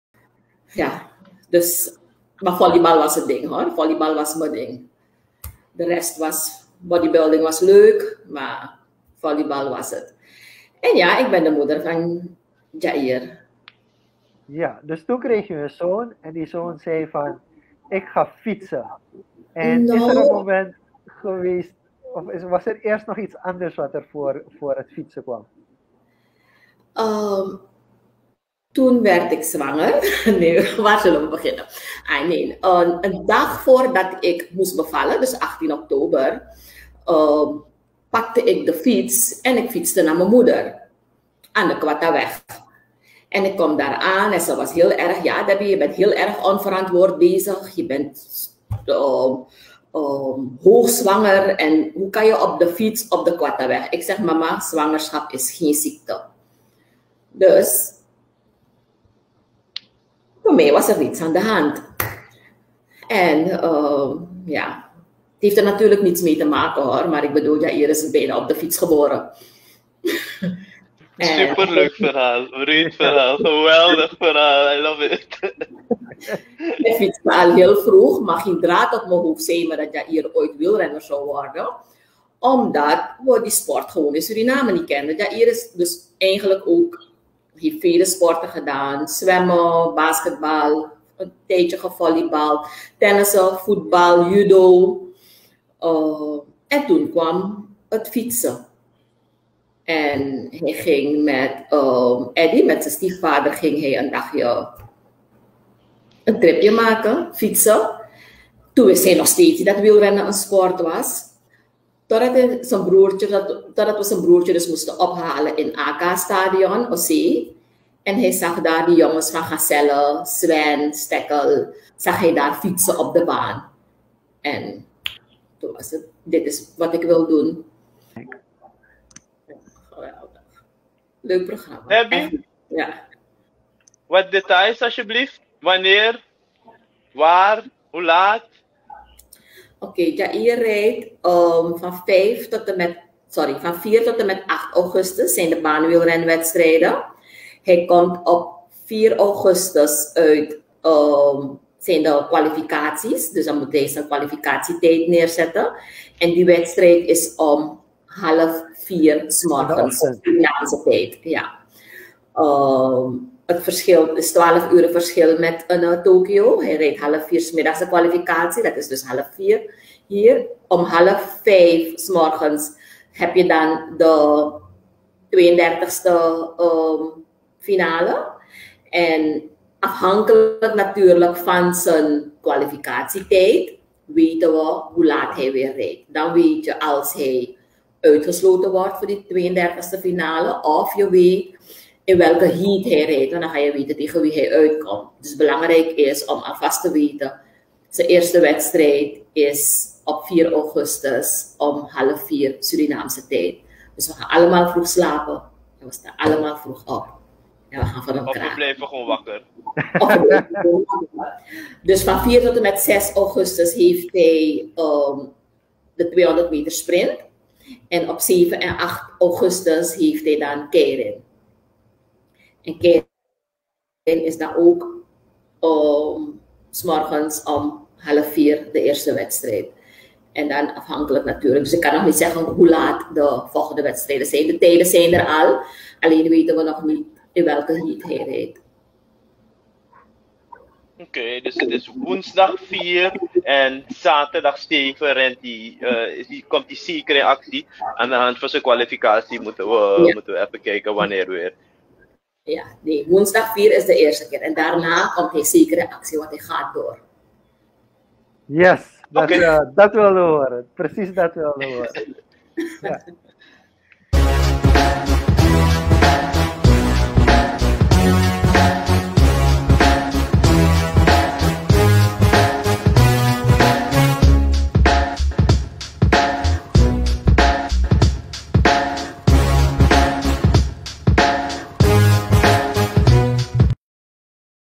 Jair Tjon en FA fietste al voor zijn geboorte
Een segment van het internview met Debora Frankel, moeder van Jair Tjon En Fa over zijn jeugd en zijn journey naar de Olympische spelen.